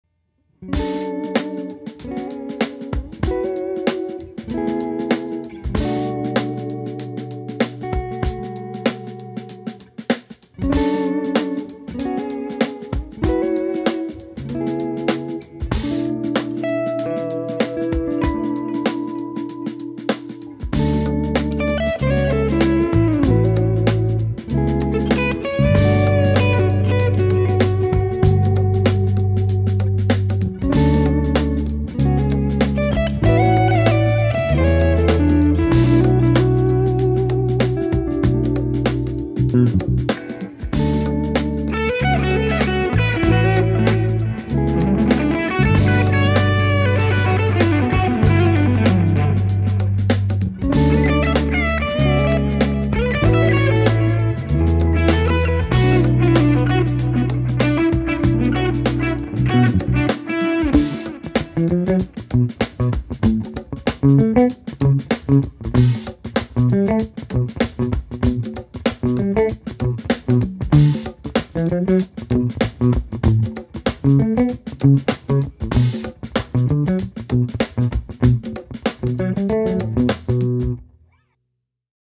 PRS Fiore Mark Lettieri Larkspur DEMO
This a limited 1 out of 10 guitar with a real signature from Mark Lettieri on the back of the headstock!